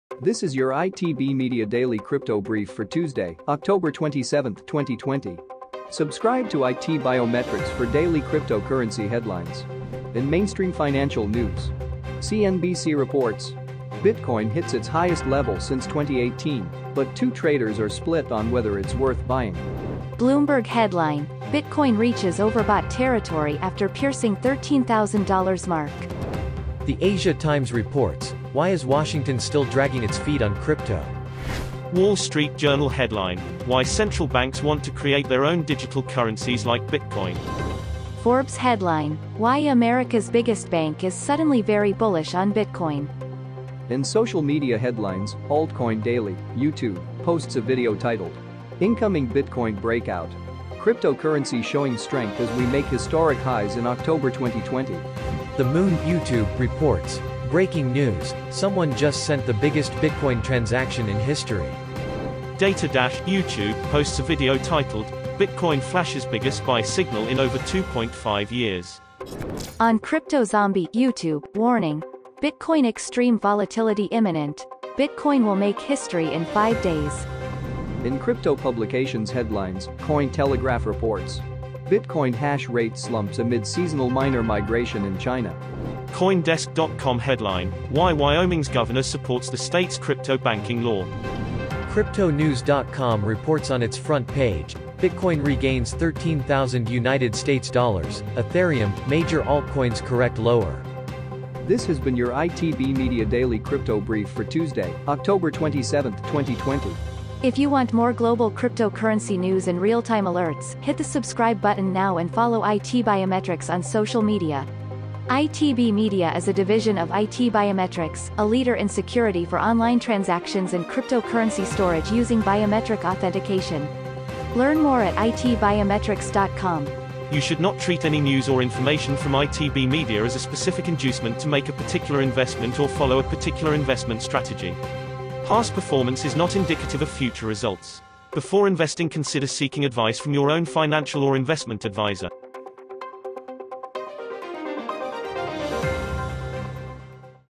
Daily Crypto Brief for Tuesday, October 27, 2020, by ITB Media
This is your ITB Media DailyCryptoBrief for Tuesday, October 27, 2020.

CNBC reports: Bitcoin hits its highest level since 2018, but two traders are split on whether it’s worth buying

Bitcoin prices climbed more than 3% to levels not seen since January 2018 on Thursday after PayPal’s announcement that it would be launching a cryptocurrency service. Up nearly 23% month to date, bitcoin also received some accolades from legendary hedge fund manager Paul Tudor Jones, who told CNBC on Thursday that the cryptocurrency was in its “first inning” and likely would be “the best inflation trade” in a time of widespread quantitative easing.

After surging past $13,000 on Wednesday for the first time in more than 14 months, price charts followed by technical analysts suggest the largest cryptocurrency may be due for a pause in the wake of the exuberance triggered by the announcement that PayPal Holdings Inc. will let customers use cryptocurrencies. The rally pushed Bitcoin’s 14-day Relative Strength Index (RSI) reading to 80, a level that chartists consider overbought. Additionally, Bitcoin is well above the upper limit of a trading range that would normally trigger a reversion to the mean, which may indicate the digital coin needs to pause before continuing to advance.

AsiaTimes Reports: Why is Washington still dragging its feet on crypto?

As Bitcoin enjoys increasing mainstream adoption, different ways to access it are also in the spotlight. While the cryptocurrency was once reserved for the tech-savy and Wall Street powerhouses, a growing number of Bitcoin ATMs (BTMs) are serving as financial tools to empower Main Street investors.

WallStreetJournal Headline: Why Central Banks Want to Create Their Own Digital Currencies Like Bitcoin

Central banks are exploring ways to create a digital version of cash: money that is trusted, convenient to use and widely available to people, for making payments and getting paid. Digital currencies have the potential to make it easier, cheaper and faster to move money around.

Forbes Headline: Why America’s Biggest Bank Is Suddenly Very Bullish On Bitcoin

The bitcoin market has matured since JP Morgan chief executive Jamie Dimon called bitcoin a "fraud" in September 2017—helped by the (still highly volatile) bitcoin price finding support as an inflation hedge alongside gold this year. Now, JP Morgan has said bitcoin's strong 2020 could be set to continue, finding the bitcoin price has "considerable" upside in the long-term as it better competes with gold as an "alternative currency."

CryptoSocial Media

Altcoin Daily (YouTube) posts a video titled: Incoming Bitcoin Breakout! Cryptocurrency Showing Strength as We Make Historic Highs in October 2020.

The Moon (YouTube) reports: BREAKING NEWS: SOMEONE JUST SENT THE BIGGEST BITCOIN TRANSACTION IN HISTORY!!

Data Dash (YouTube) posts a video titled: Bitcoin Flashes Biggest Buy Signal In Over 2.5 Years.

On Crypto Zombie (YouTube): WARNING!!! BITCOIN EXTREME VOLATILITY IMMINENT!!! BTC WILL MAKE HISTORY IN 5 DAYS!

Cointelegraph reports: BTC hash rate slumps amid seasonal miner migration in China

On Oct. 26, Thomas Heller of Bitcoin (BTC) mining blog Hashr8 reported that roughly 22 exahashes per second (EH/s) of mining power had left the Bitcoin network, coinciding with the end of the season the previous day (based on weather forecasts).

That’s when Kraken Financial became the first entity to receive a special purpose depository institution (SPDI) charter in the Cowboy State, giving the cryptocurrency industry insight into roughly how long it takes to become a bank. It’s also the first newly chartered (de novo) bank the state has approved since 2006.

Cryptonews reports on its front page: Bitcoin Regains USD 13,000, Ethereum, Major Altcoins Correct Lower

After a short-term downside correction, bitcoin price found support above USD 12,750. BTC started a fresh increase and it is currently (05:00 UTC) trading above USD 13,000. However, the price must settle above USD 13,200 and USD 13,250 to continue higher in the near term.

This has been your ITB Media Daily Crypto Brief for Tuesday, October 27, 2020.